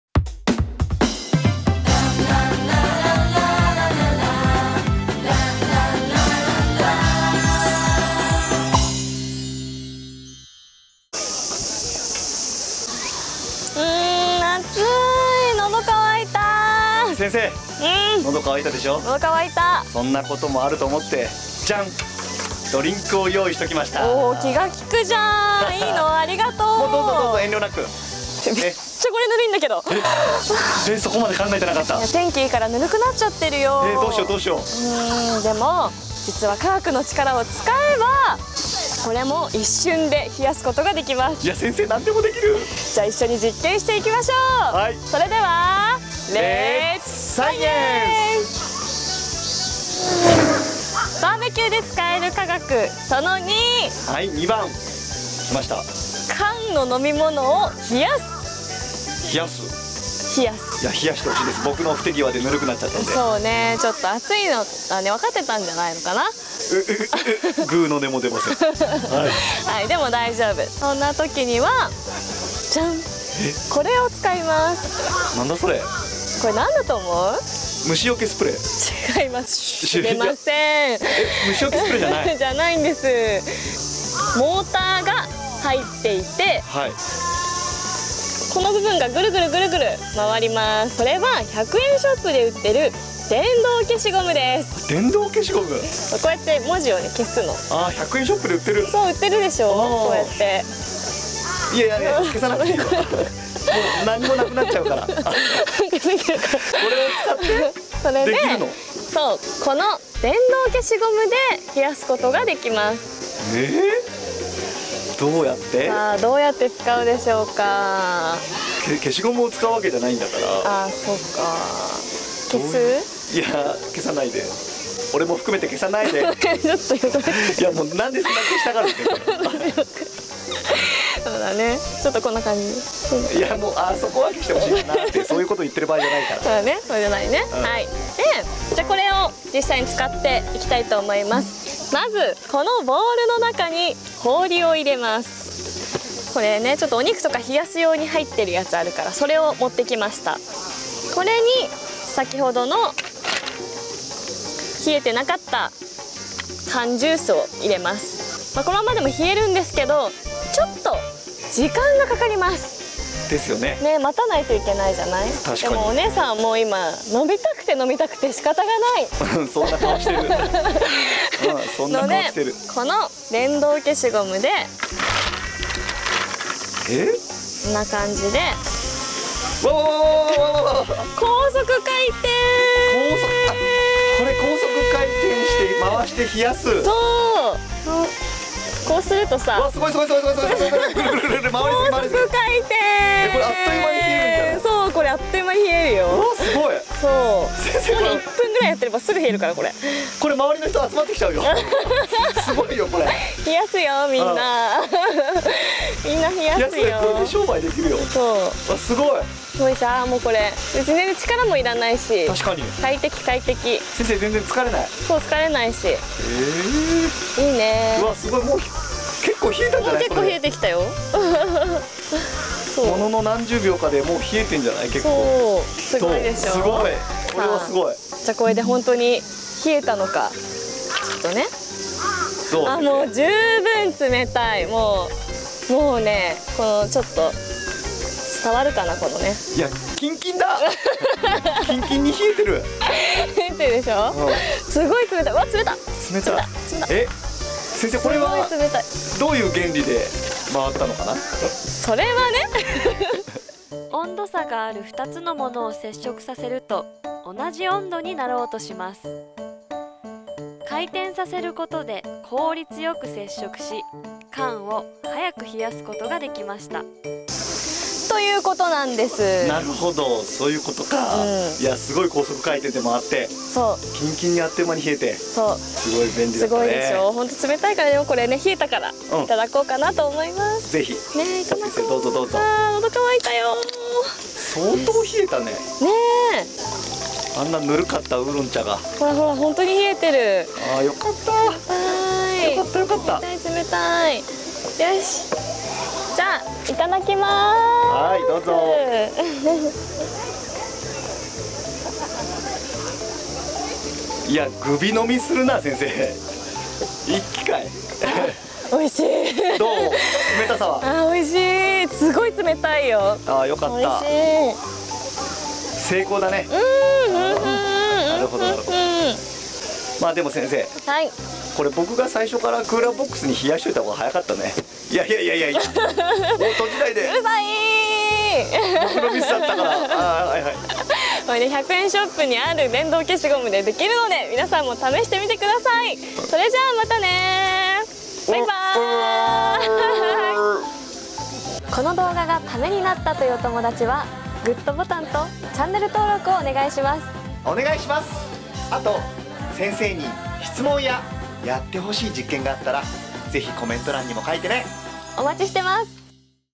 【科学の力でBBQ②】ぬるい飲み物を一瞬で冷やす？！【スペシャル実験】
今回もミキラボスペシャル！！
BBQを科学の力でもっと楽しく盛り上がろう♪
第2回は、ぬる〜い飲み物を一瞬で冷やす夢のような実験です。
みんなもいっしょに、Let's Science‼︎

0:00 オープニング
0:50 缶を冷やす方法！
4:13 冷えるメカニズム！
4:27 まとめ！

▶︎アスキーキッズもチェック！